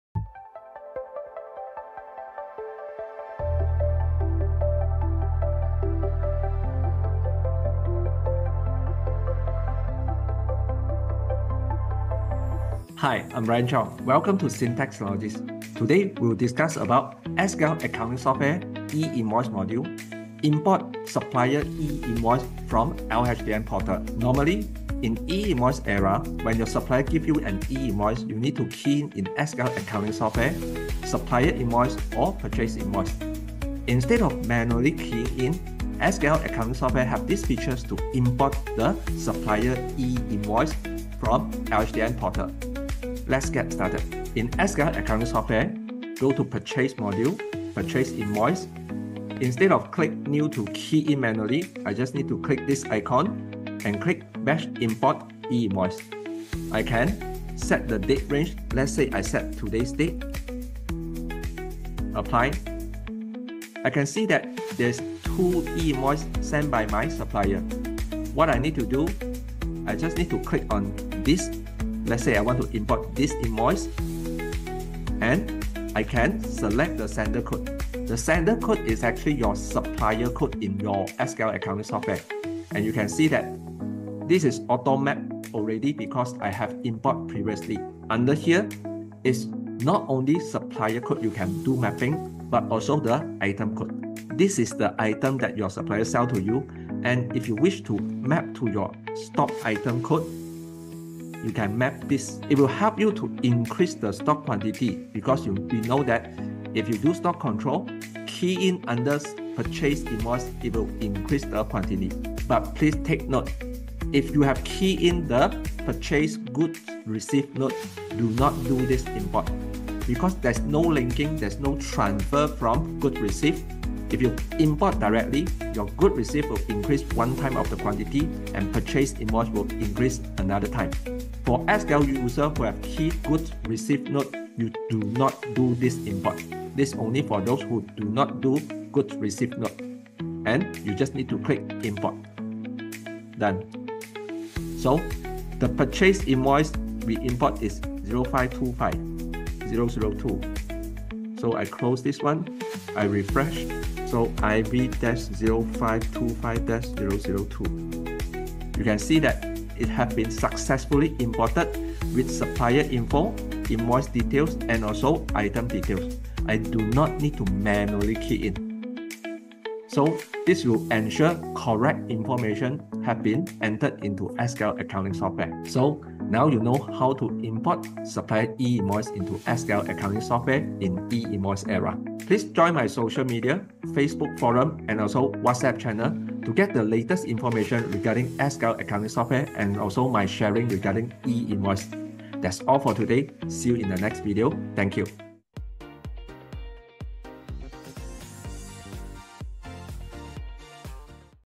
How to Import Supplier E-Invoice into SQL Accounting System as Purchase Invoice?
Hey everyone! This video will show you How to Import Supplier E Invoice into SQL Accounting System as Purchase Invoice in SQL Accounting software😆. Follow this step-by-step guide!